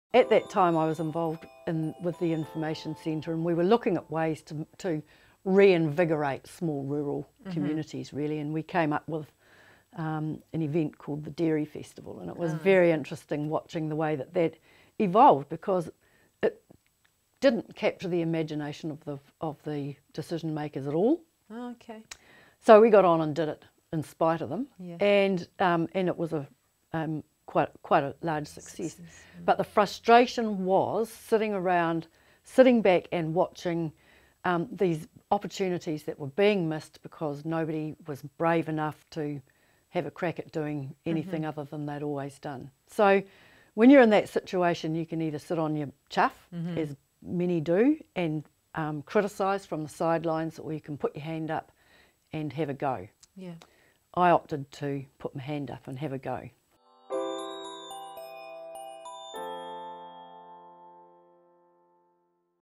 Mary Bourke - Community Leader | Trailer
Mary Bourke is the original disruptor and glass ceiling breaker, becoming the first female Mayor of South Taranaki and successfully running the district for five terms (15 years), providing an amazing example of leadership and installing positive aspirations in a generation of young provincial girls. Mary’s community and governance roles grew from her Mayoralty to include Chairing the TDHB and WITT Board. Today Mary is an independent Chair, Facilitator, Presenter and Celebrant, working within her business Mary Bourke Unlimited.